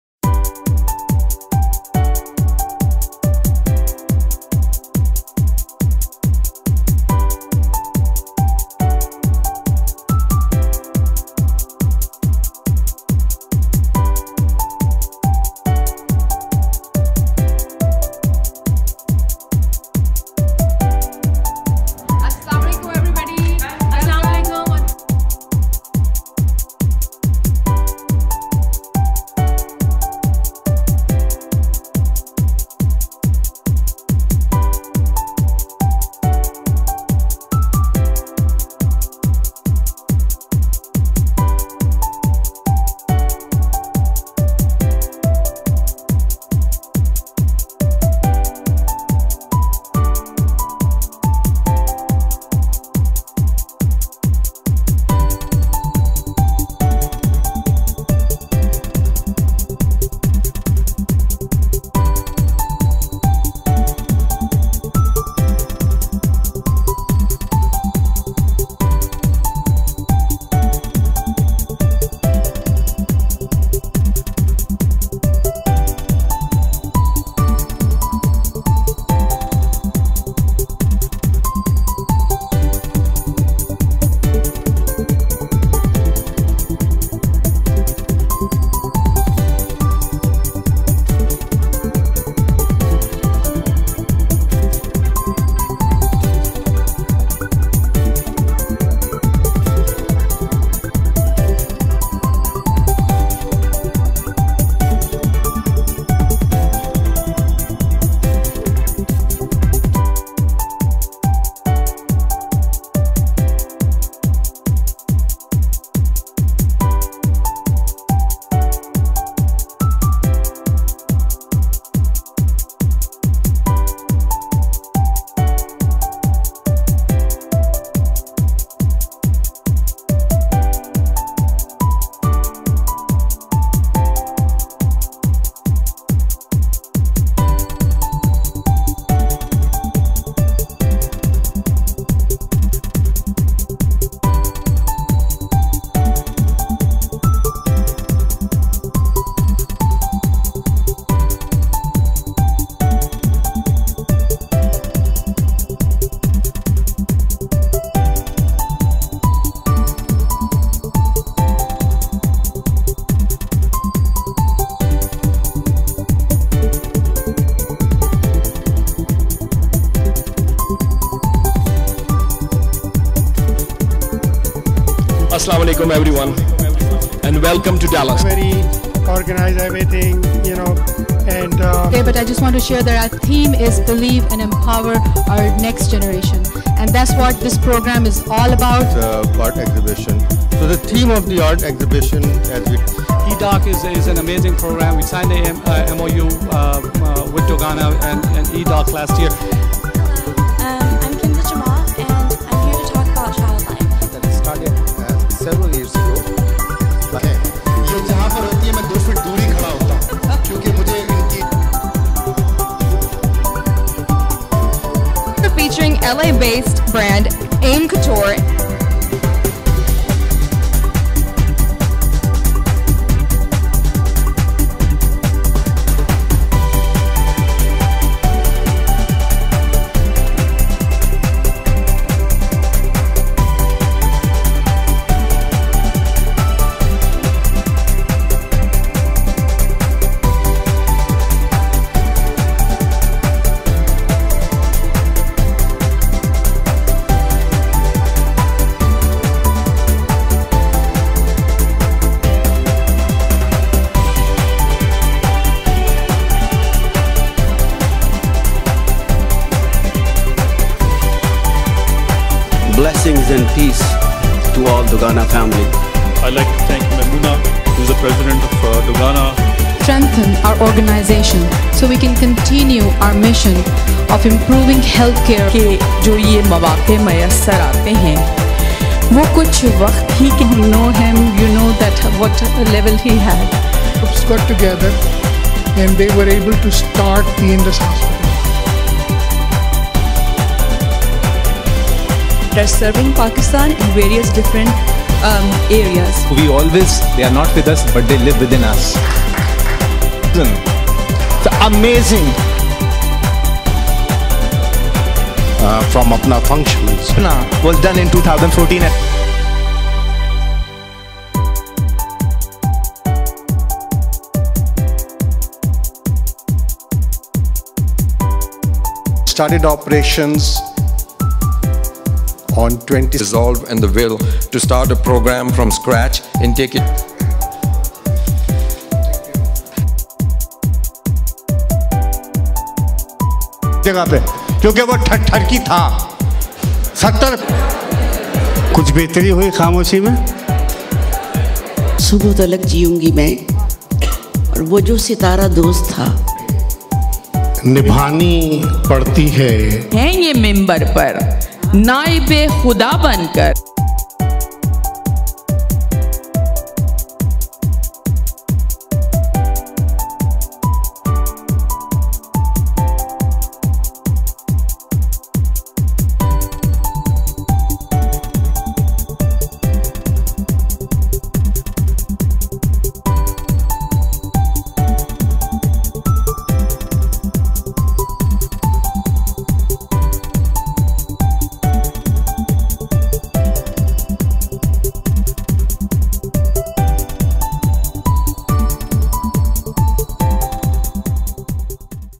Dogana Spring Meeting Dallas - Highlights
This video is about Doctors of Dow Graduates From  North America.
Long Live Humanity